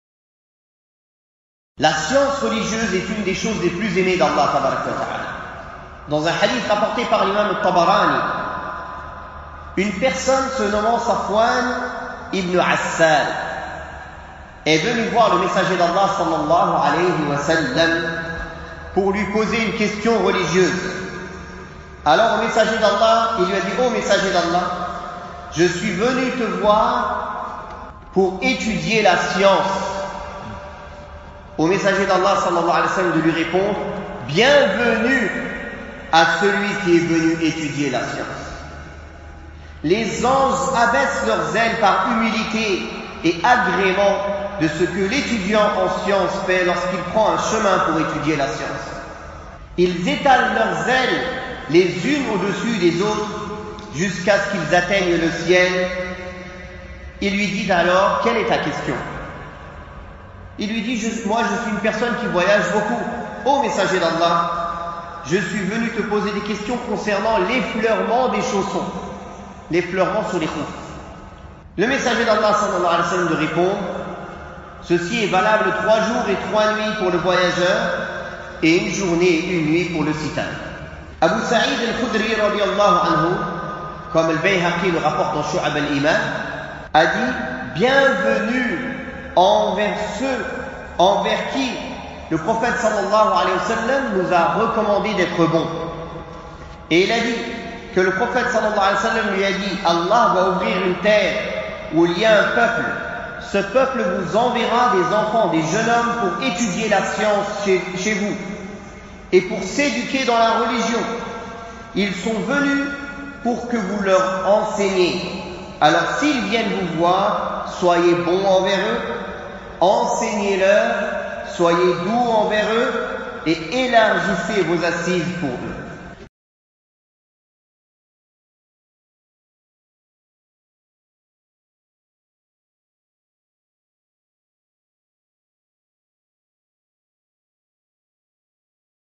Bienvenue à celui qui est venu demander la science (Abou Laïth 'Othmân)
Extrait #1194 Bienvenue à celui qui est venu demander la science

L'apprentissage de la science, le seul chemin pour concrétiser la servitude à Allah.
Conférence du Vendredi 19/05/1440 (25/01/2019) au Masjid Al-Rahim à Dubaï
(organisée sous l’autorité du département des affaires islamiques de Dubaï sous l’autorisation n. 924/2018.)